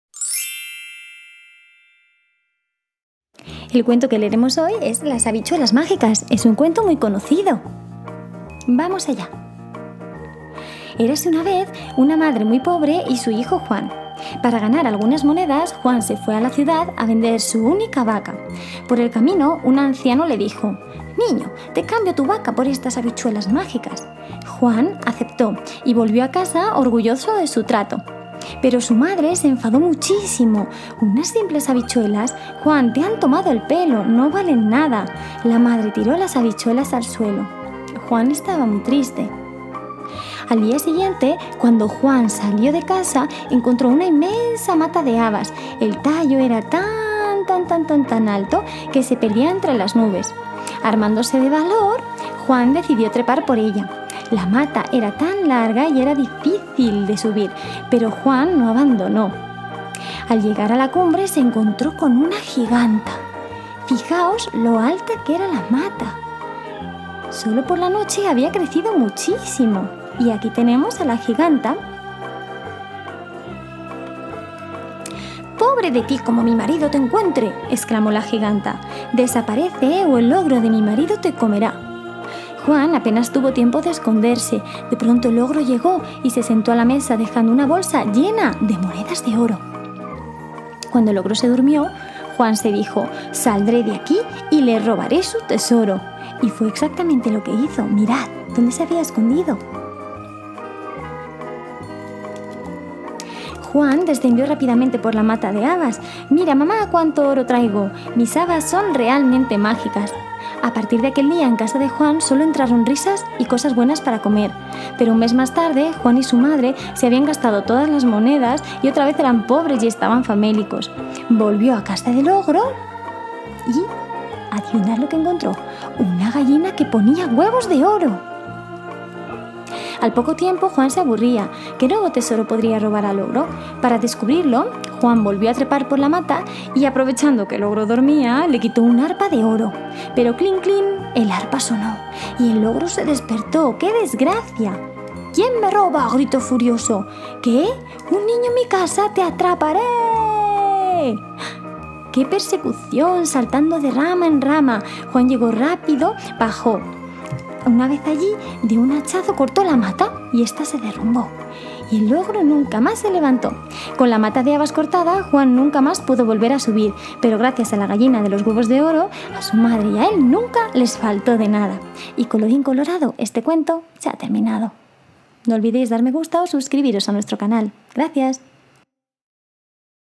Cuentos infantiles: Las Habichuelas mágicas libro infantil en español
El clásico cuento de las habichuelas mágicas. Disfruta de las aventuras que vive Pedro cuando descubre que sus habichuelas son mágicas.